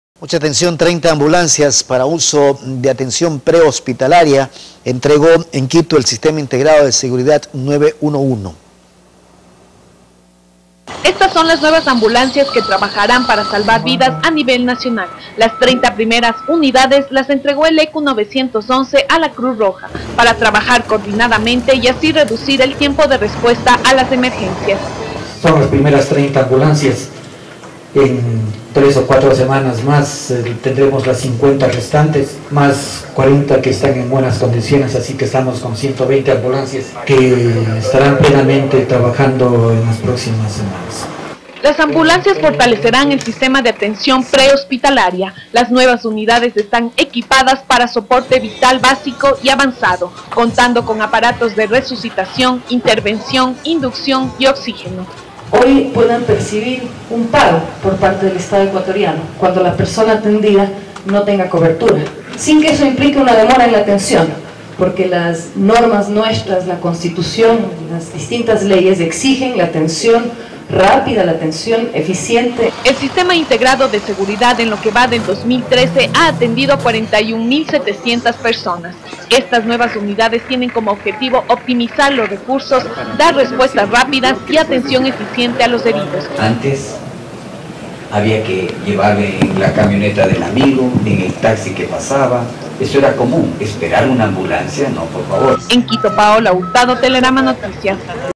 ECU 911 entregó 30 ambulancias
30 ambulancias para uso de atención pre hospitalaria,  entregó en Quito  el sistema integrado de seguridad  ECU 911 por parte de la Cruz Roja ecuatoriana..

Las unidades   serán distribuidas  a  nivel  nacional..